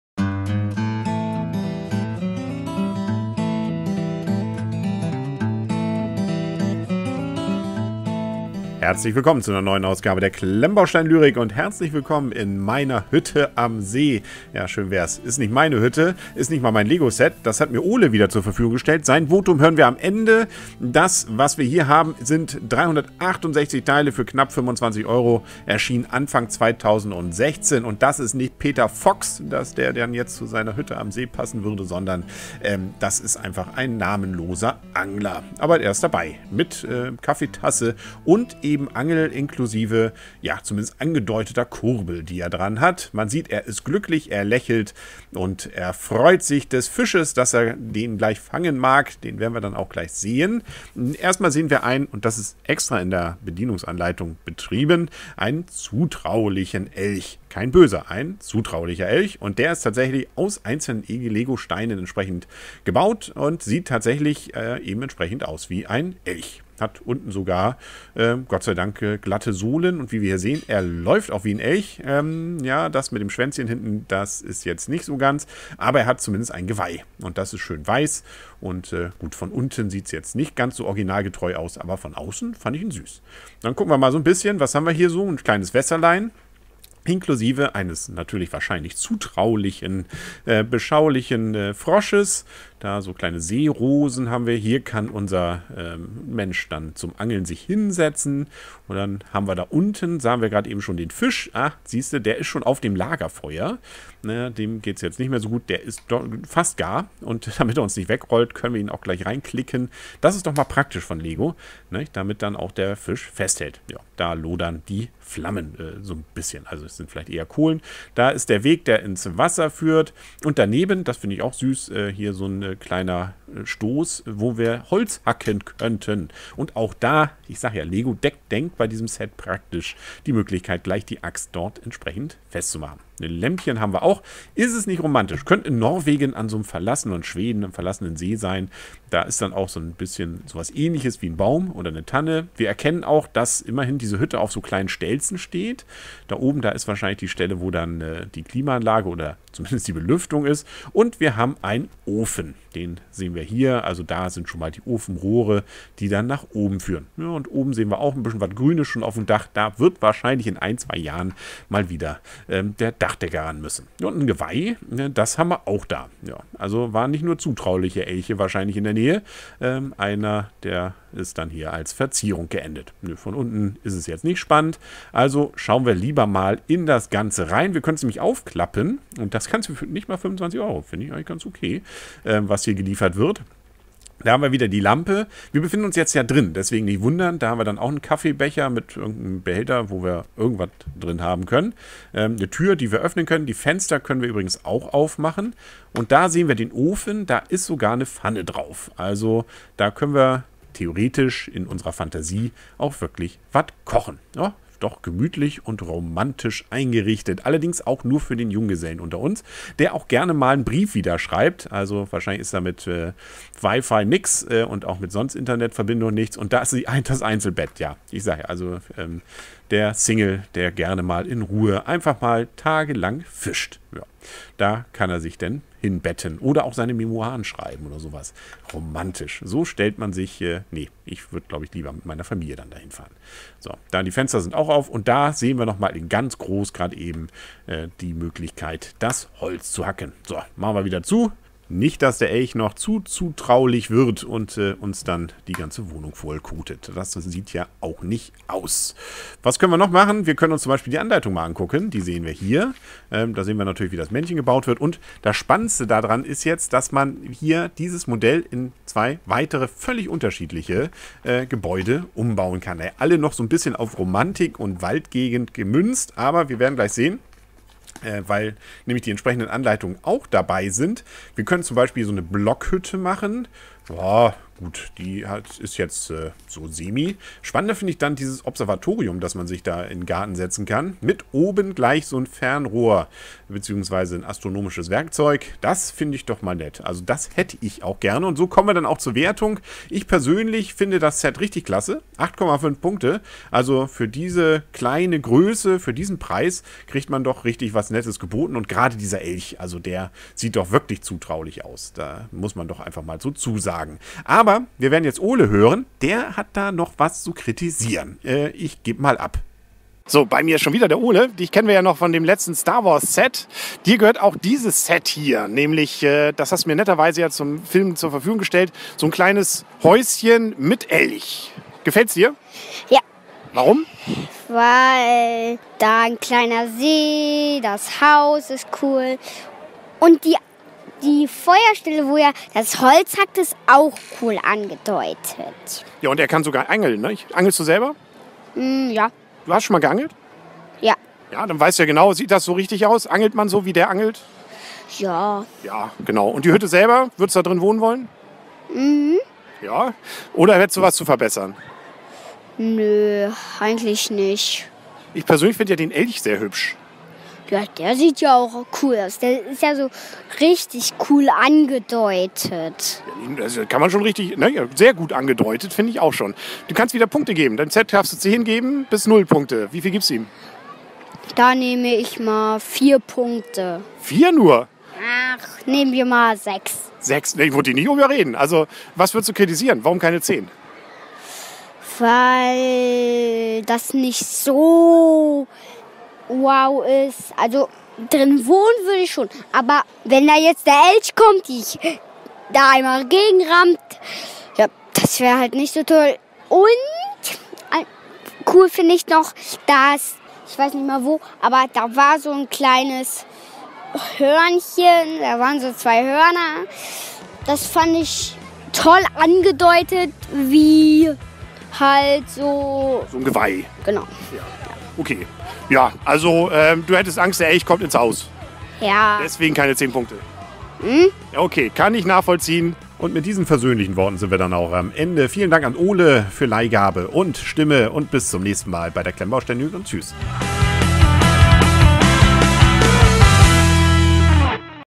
LEGO Hütte am See (Test Set 31048 Review deutsch der Lakeside Lodge)
Danke wieder an Ole für die Leihgabe seiner "Hütte am See" (Set 31048). Diese "Lakeside Lodge" beinhaltet nicht nur alles, was so ein Junggeselle auf seiner Angeltour braucht, sondern auch einen Elch. Bei der Bewertung des Sets sind sich Ole und Henry (Caulius) jedoch nicht einig ...